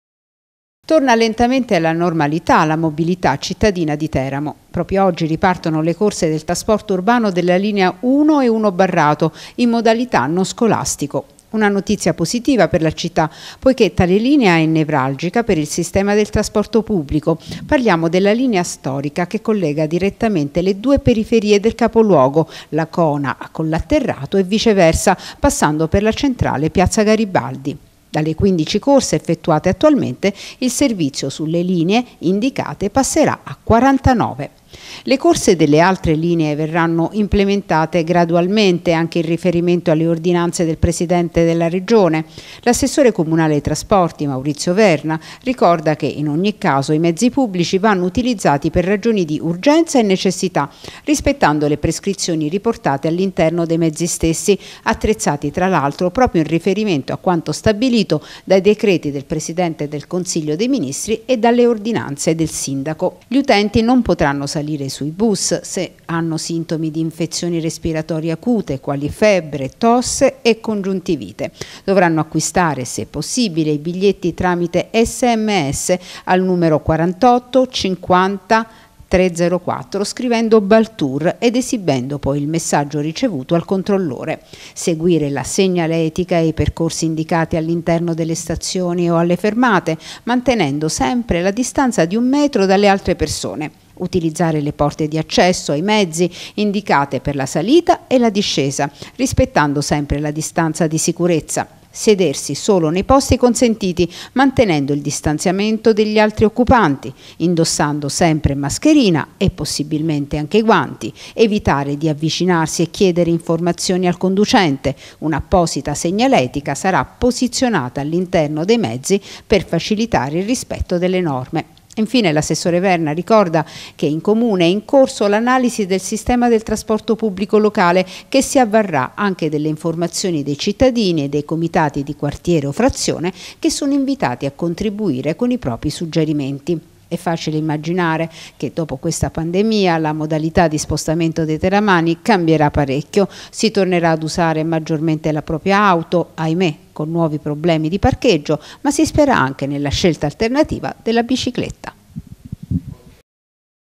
Il Comune riattiva le linee principali dei bus. Biglietti con Sms
Il Comune di Teramo ha riattivato da oggi la linea 1 del bus urbano che collega la città da un capo all'altro, passando per il centro cittadino. I biglietti potranno essere acquistati con Sms. Intanto, l'amministrazione sta studiando un piano definitivo del trasporto pubblico locale e invita i residenti a inviare suggerimenti in base alle loro necessità.